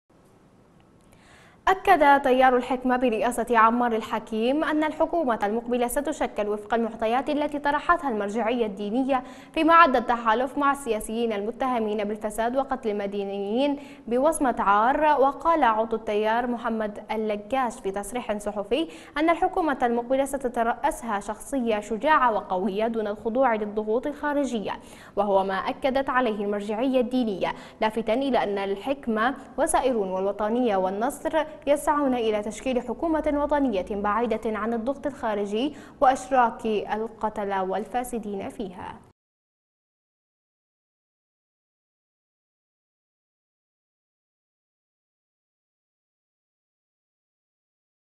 الحكومة ستشكل وفق المعطيات التي دعت اليها المرجعية الدينية
مواصفات تشكيل الحكومة المقبلة

أكد تيار الحكمة برئاسة عمار الحكيم أن الحكومة المقبلة ستشكل وفق المعطيات التي طرحتها المرجعية الدينية، فيما عد التحالف مع السياسيين المتهمين بالفساد وقتل المدنيين بوصمة عار. وقال عضو التيار محمد اللكاش في تصريح صحفي إن الحكومة المقبلة ستتراسها شخصية شجاعة وقوية دون الخضوع للضغوط الخارجية , وهو ما اكدت عليه المرجعية الدينية , لافتا إلى إن الحكمة وسائرون والوطنية والنصر يسعون إلى تشكيل حكومة وطنية بعيدة عن الضغط الخارجي واشراك القتلة والفاسدين فيها .